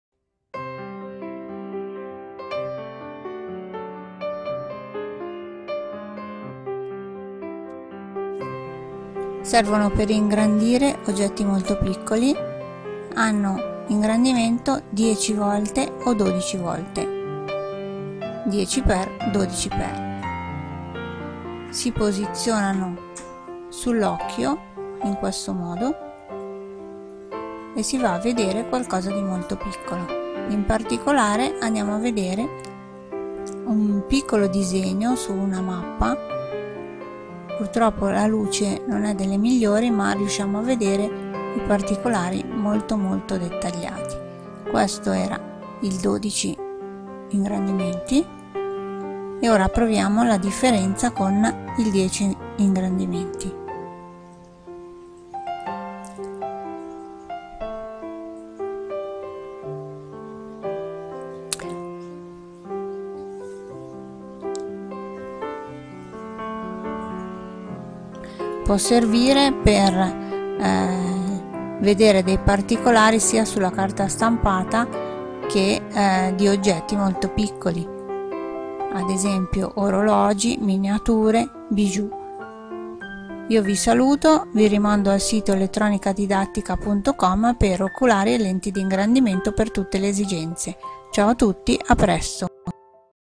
Oculari da orologiaio e da orafo. Oculari 10x e 12 per osservare piccoli oggetti da collezione
Ecco due oculari da orologiaio con ingrandimento 10x e 12x. Oculari per gemme e orologi, oggetti da collezione. Questi oculari possono essere usato anche per le scienze naturali per vedere insetti, fiori, foglie, minerali e fossili.
Lente oculare per osservare in modo preciso e nitido.
La lente oculare è anche chiamata monocolo.